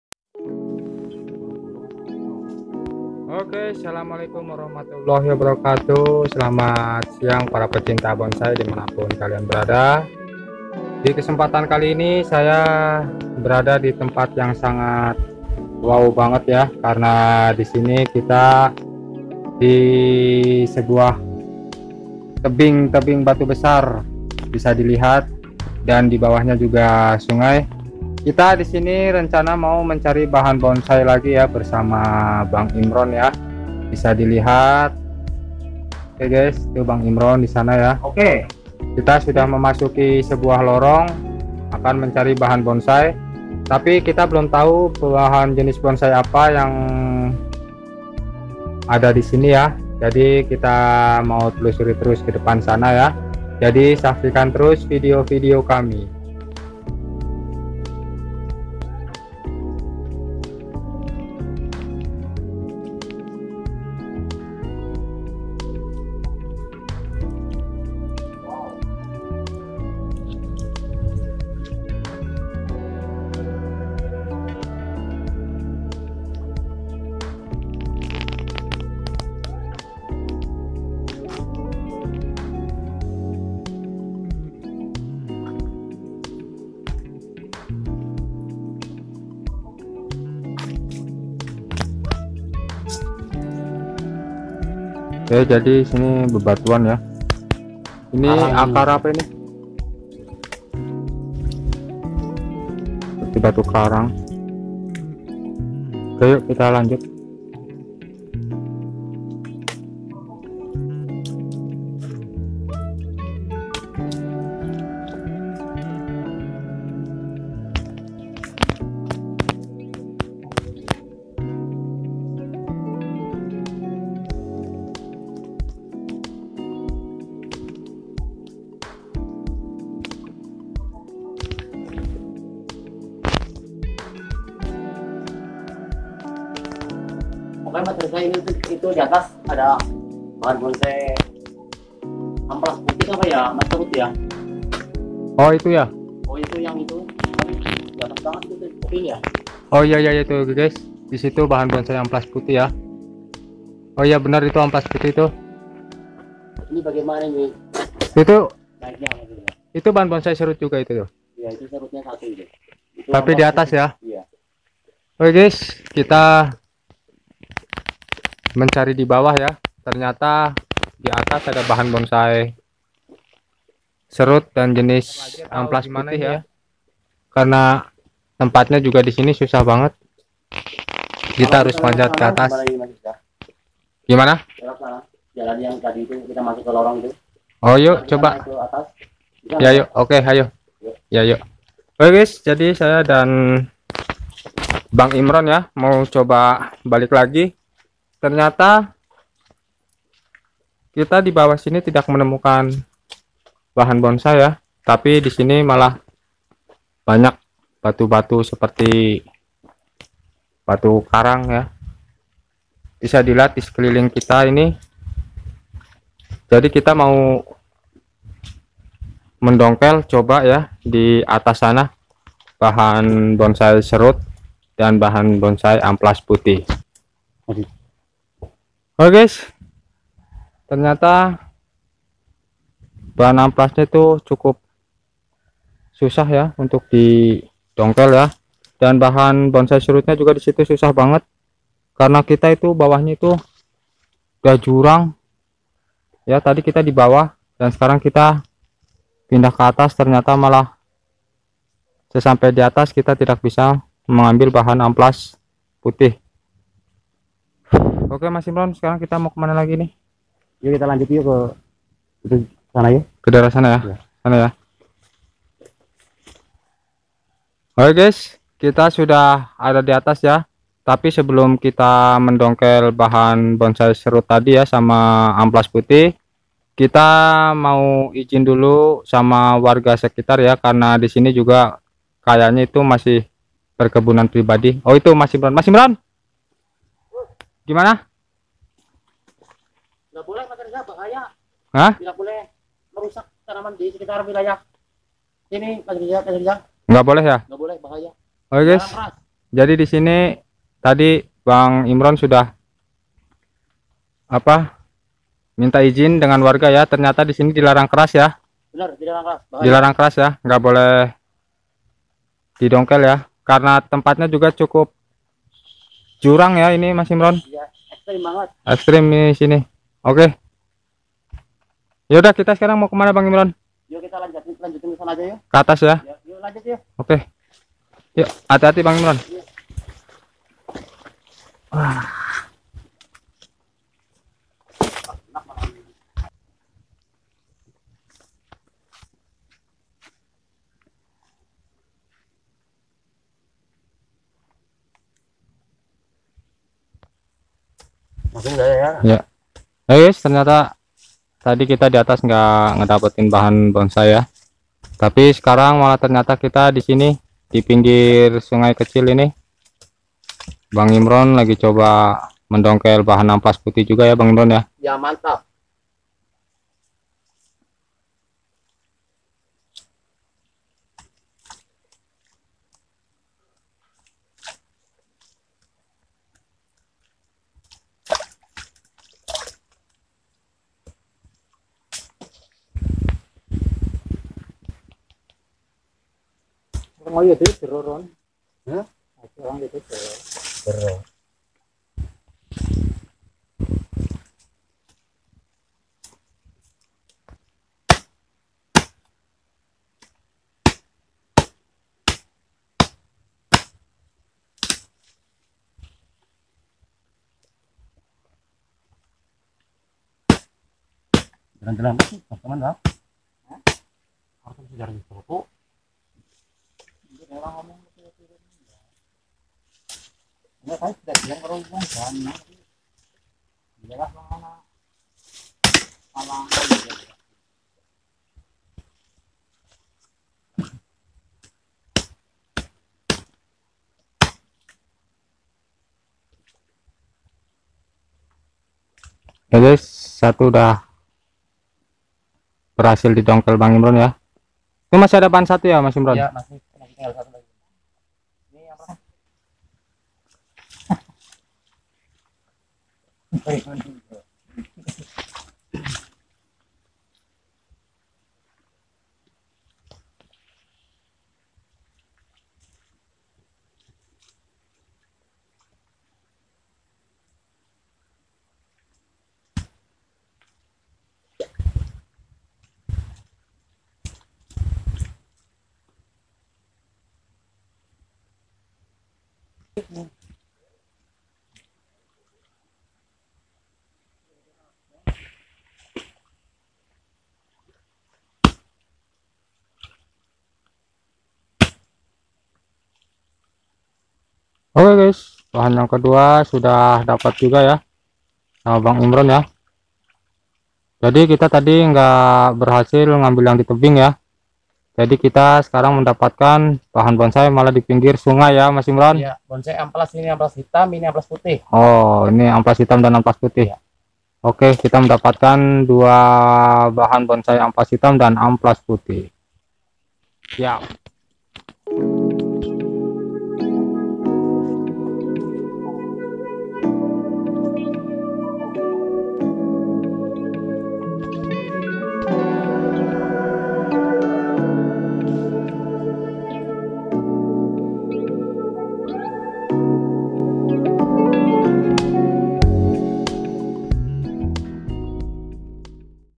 || Habitatnya Pohon Ampelas PutiH Dan Hitam_
Habitatnya Pohon Amplas PutiH Dan Hitam_ Reza tv

Assalamualaikum slamat siang pecinta bonsai, kolektor bonsai, penghobi bonsai, dan sedulir suscreber di manapun kalian berada , kali ini saya sedang mencari Bahan bonsai ,, Alhamdulillah saya mendapat kn 2 jenis Amplas Hitam dan putih . Walaupun kita melihat ada jenis Bahan serut, Tapi Seketika kita izin kepada pemilik , ternyata di larang keras untuk mendongkel Bahan bonsai tersebut. Karena Area Bahan bonsai Amplas putih dan Bahan bonsai serut , Ada di atas tebing ..